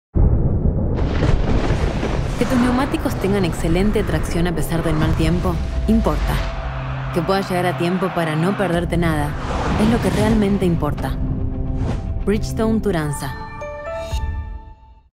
Bridgestone Turanza T005 | Manejá seguro en la lluvia
Descubrí una experiencia de manejo suave, silenciosa y confiable. Conocé las llantas Bridgestone Turanza T005
Absorben el ruido del camino y te dan un excelente rendimiento tanto en condiciones secas como mojadas.
Elegí Bridgestone Turanza para tu auto.